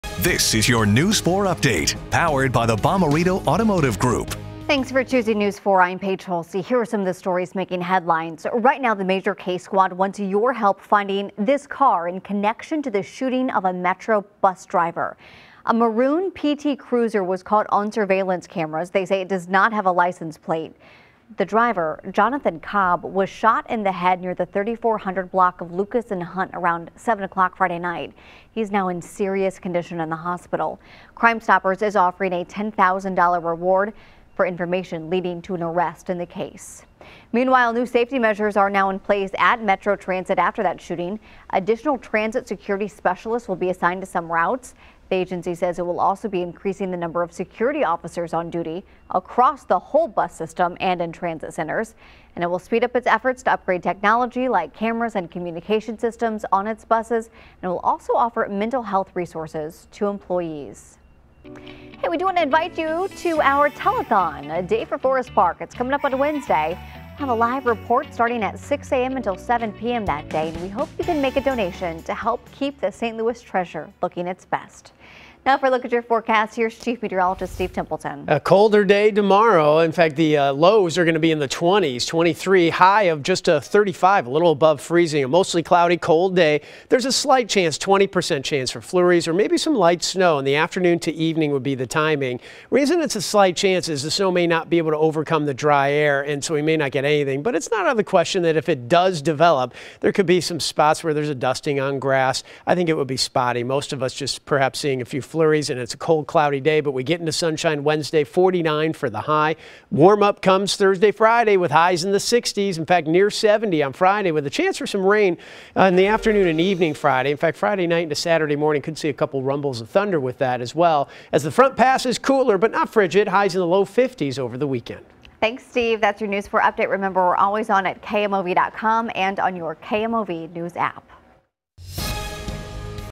News 4 Evening update: December 6, 2020
A reward is being offered for information that leads to an arrest in the shooting of a Metro bus driver, enhanced security on Metro buses start, the Forest Park Forever telethon is coming up and colder weather sticks around Tuesday